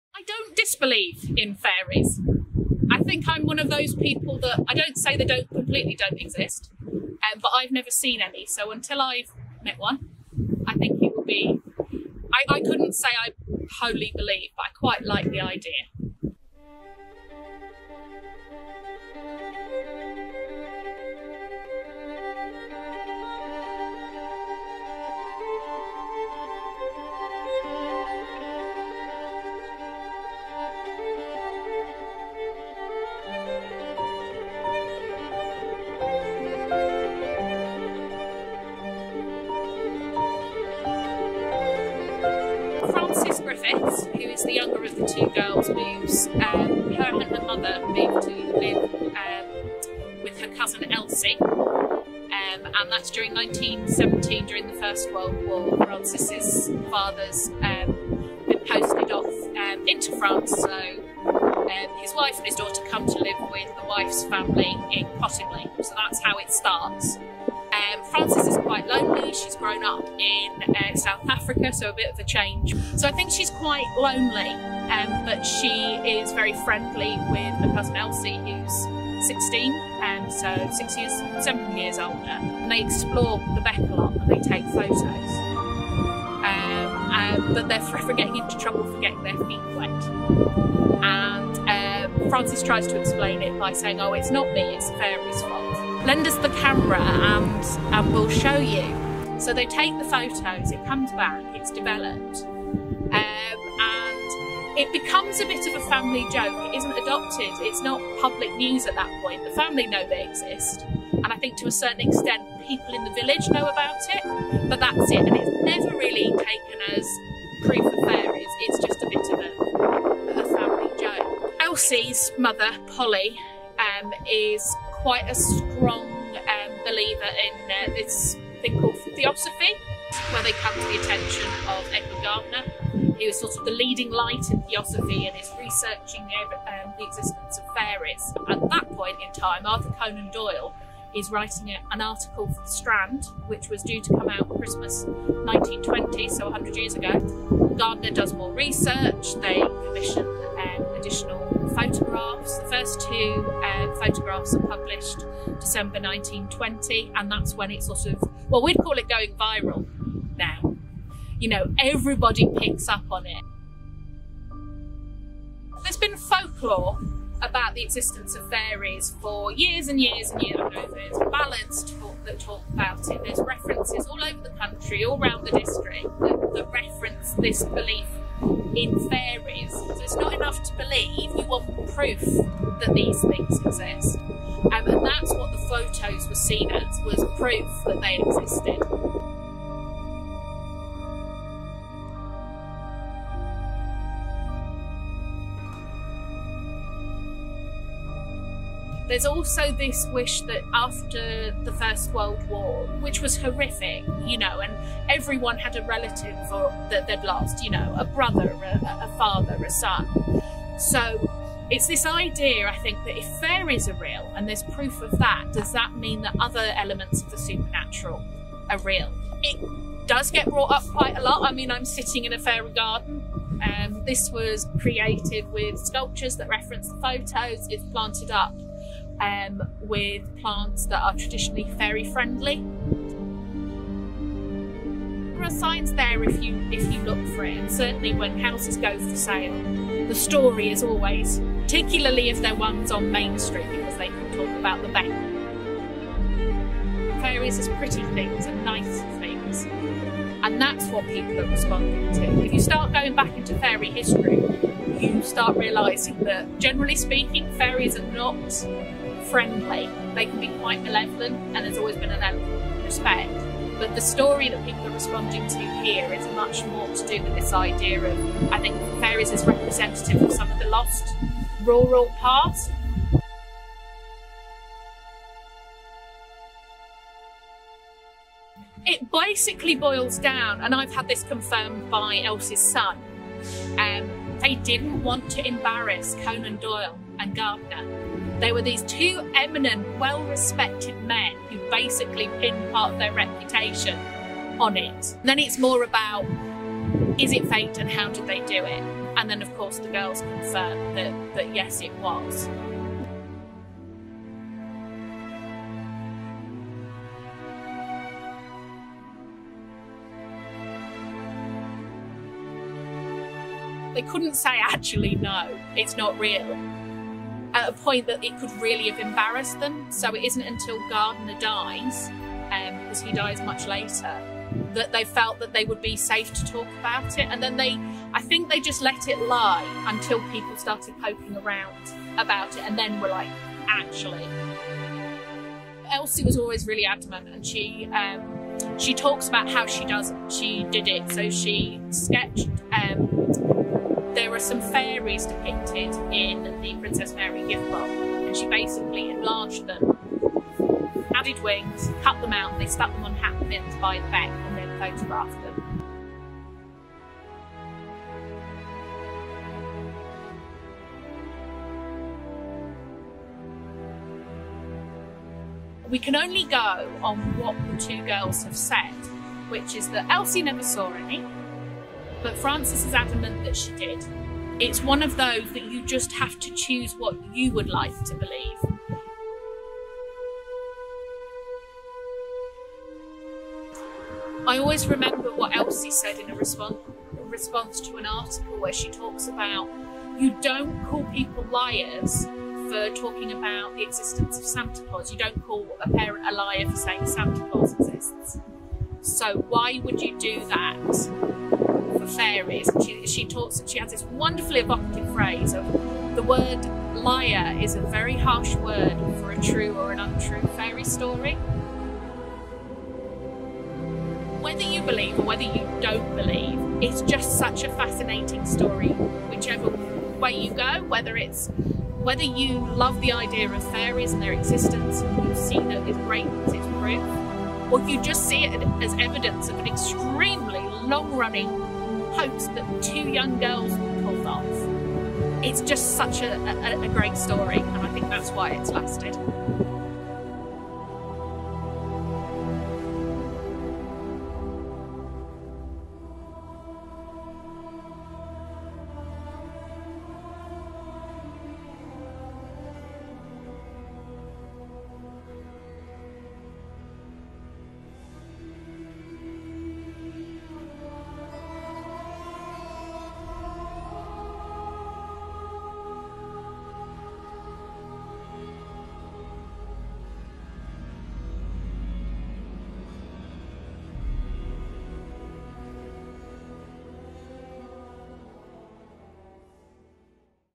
away with the fairies - a short documentary
a short documentary about the cottingley fairies and how the event came to be one of the biggest hoax in the 21st century.